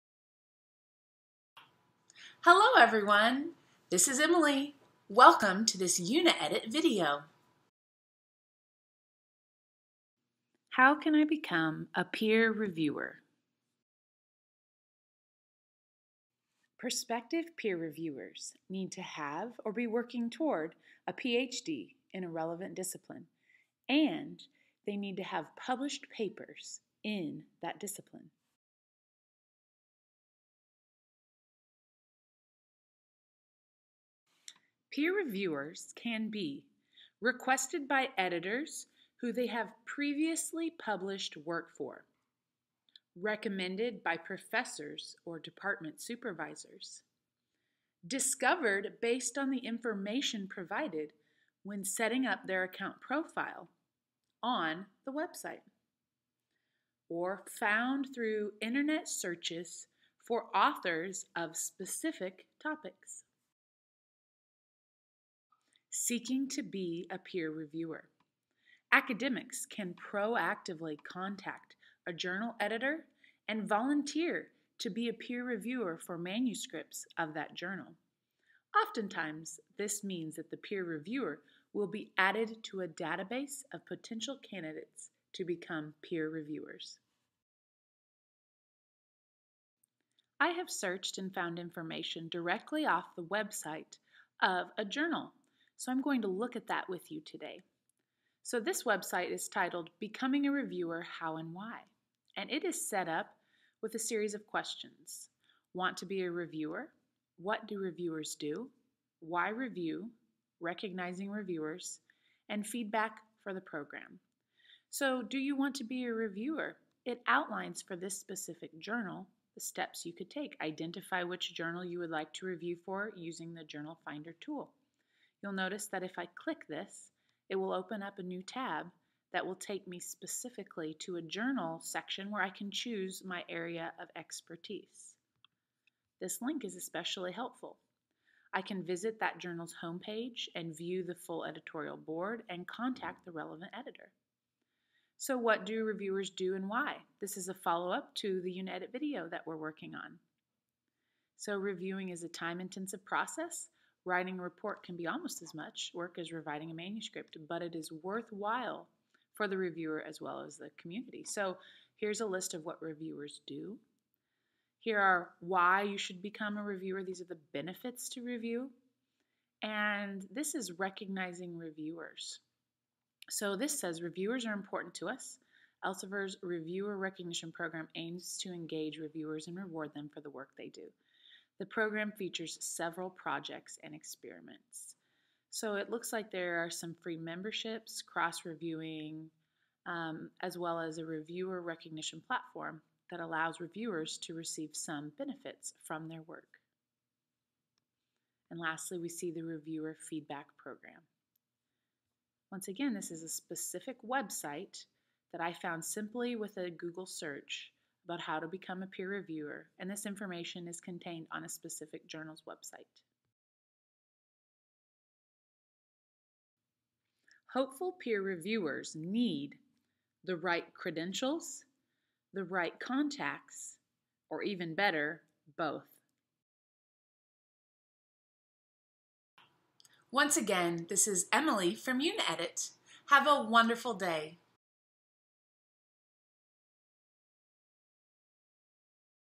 How can I become a peer reviewer?
Become a peer reviewer in the academic publishing word or build a paper reviewer career.

CHAPTERS

0:00 Intro - How can I become a peer reviewer
0:13 Qualifications for becoming a peer reviewer
1:05 Seeking to be a peer reviewer.
1:28 Peer reviewer career
3:34 What hopeful peer reviewers need

SCRIPT OF VIDEO

How can you become a peer reviewer?

Prospective PRs need to have—or be working toward—a PhD in a relevant discipline, and they need to have published papers in that discipline. Most academics become a PR when a journal editor invites them to review a paper for their journal. However, this can happen in several different ways. The most common requests for PRs come from editors of journals in which the PR has previously published. However, requests can also result from recommendations from professors and department supervisors. For example, Stephen recounted that he was invited to review a paper after a professor within his department, who was also a journal editor, noted that a submission he had received was aligned with Stephen’s research area. In another instance, the chair of Stephen’s department received an email request from a journal editor asking for PRs; thus, Stephen was recommended as someone who could review submissions pertaining to the topic. Sara explained a case in which a journal editor found her details on her departmental website; he had been looking at the website because he knew that her supervisor worked on similar subject material to that of the paper in question.

While many PRs receive requests based on a connection with either the supervisor or professor, this is not always the case. According to Sara, journal editors can sometimes identify suitable PRs based on the information academics provide about their specific subject areas when setting up their author accounts on the journal’s website. Journal editors may also search the Internet for authors of particular topics; for example, Sara was asked by a journal editor, who was previously unknown to her, to conduct a PR on a very specific subject about which she had previously published a paper. Additionally, Geraldine, who also works as a journal editor, reported that “an academic can approach a journal editor to offer their services as a PR, and they will be added to a database of potential PRs.” Therefore, you can become a PR in a variety of ways providing you have the right credentials, contacts, or even better, both.

UNI-EDIT SERVICES FOR AUTHORS

Uni-edit specializes in language services for academics and researchers.

PUB015